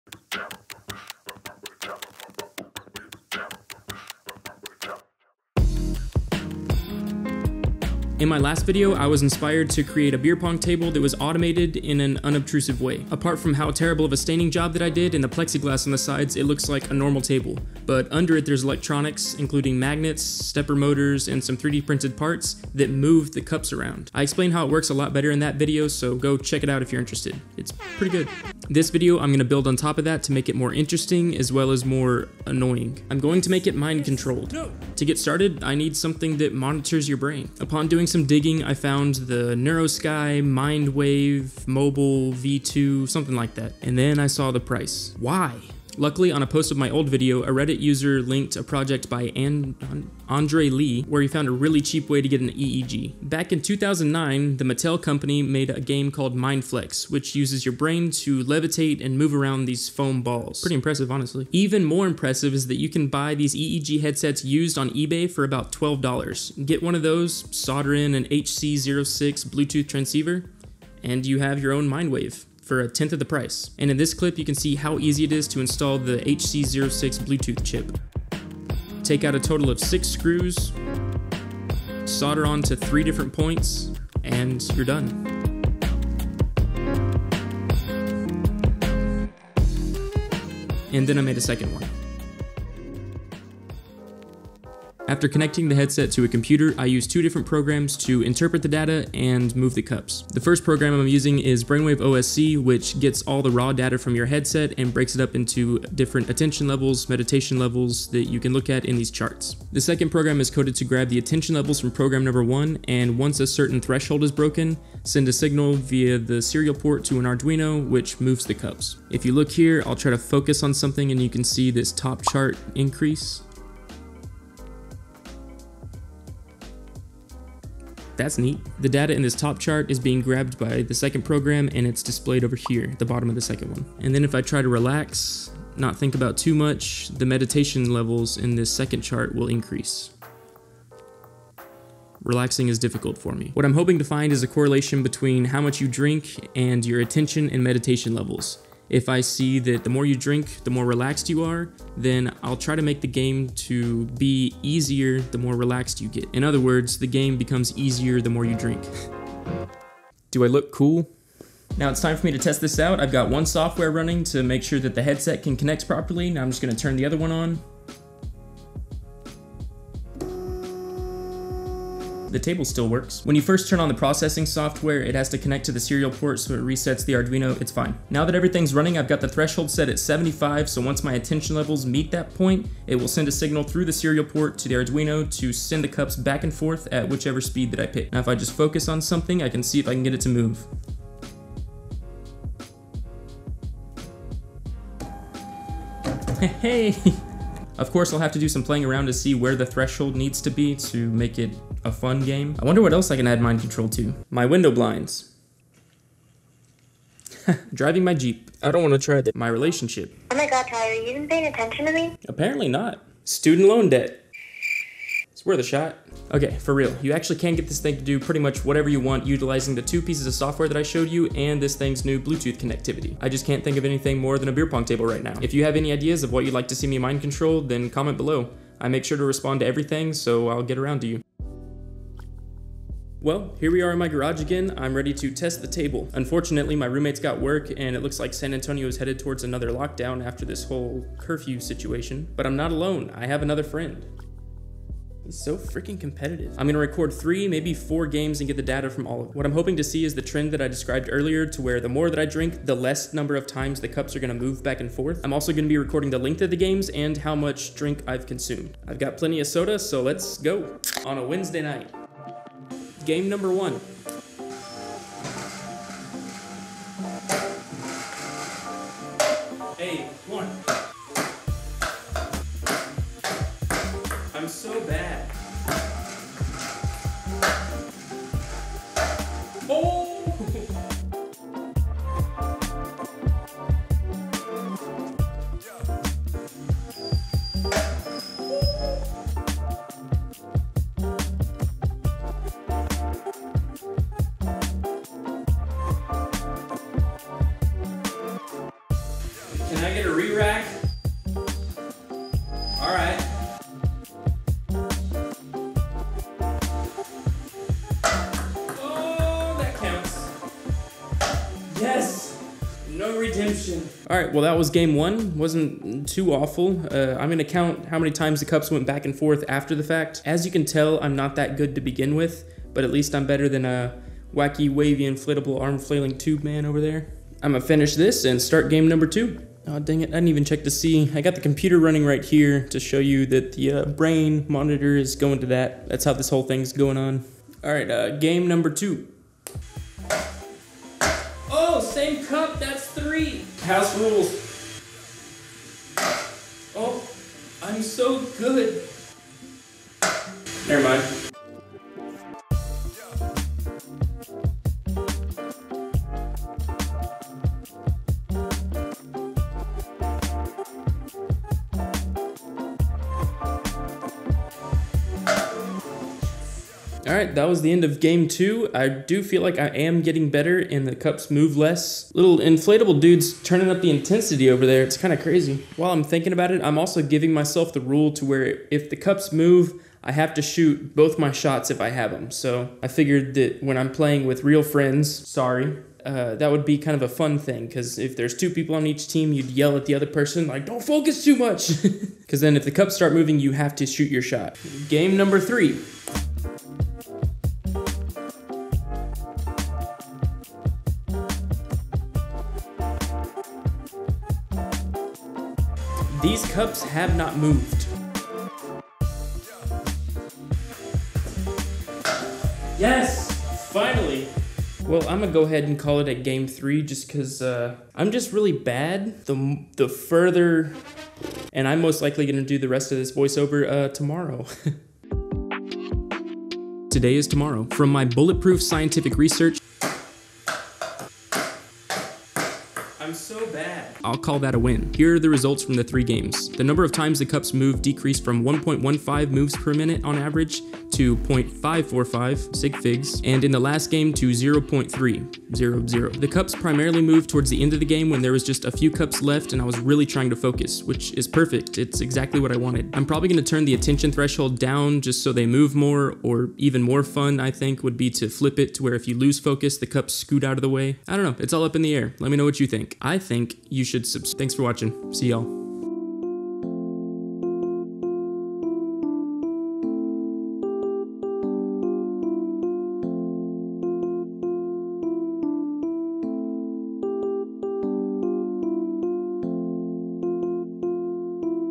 I Made Mind Controlled Beer Pong
I added Mind Control to my previous Automated Beer Pong game!

Using an EEG Headset (used MindFlex headset game purchased on Ebay for ~$12), HC-06 Bluetooth chip, and good old Arduino, the project came to life.

Once the Bluetooth transceiver is wired in and the device is paired to a computer, the Brainwave OSC software breaks the incoming data up into separate attention and meditation charts. A second program, Processing, runs a code that grabs the attention levels from Brainwave OSC. In the Processing code there is also a variable Attention threshold that once broken, sends an "on" signal to the connected Arduino. The Arduino then moves the cups at the users' selected speed.

Arduino Nano
HC-06 Bluetooth
Rotary Encoder
Stepper Motor
Stepper Motor Driver
Timing Belt kit
Limit Switch
AL2020 Extrusion Rail
Sliding Shuttle for Rail
STRONG Magnets
UHMW Tape
Table Toggle Clamp
Locking Table Legs

Timestamps:
0:00 - Intro/Concept
0:30 - How Mind Control?
1:06 - Headset Assembly
1:33 - Hardware Communication and Software
2:38 - Testing Headset with table
3:52 - Full Testing
4:21 - Game 1
5:37 - Game 2
6:34 - Game 3
7:04 - Results & Conclusion